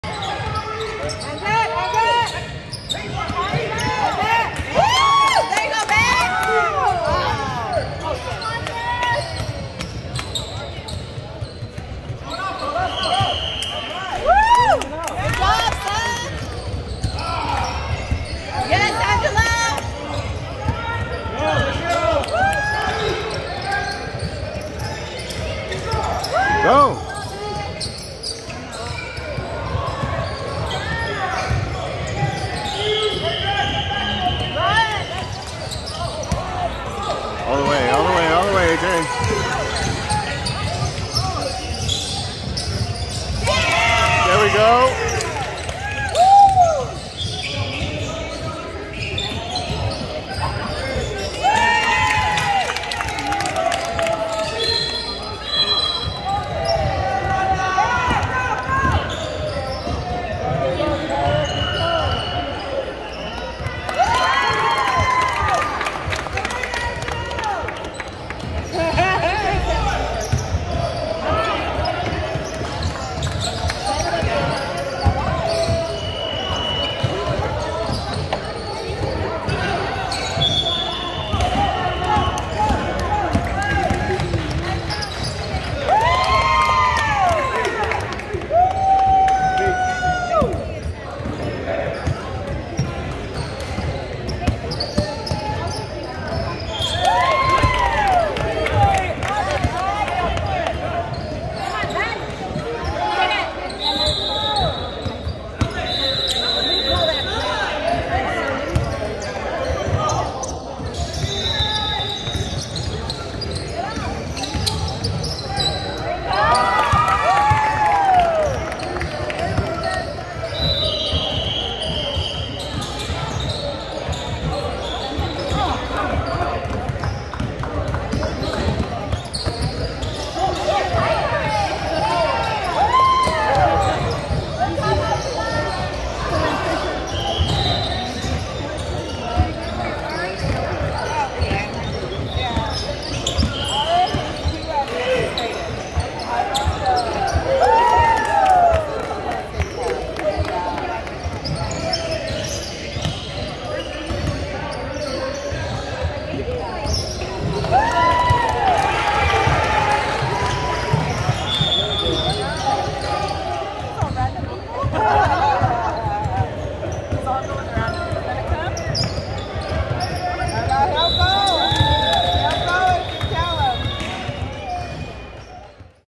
4 22 23 Highlights
Had an off day shooting, but more than made up for it with defense and facilitating. There was a point when the other teams stopped bringing the ball to his side of the court and played half the court 4 vs 4.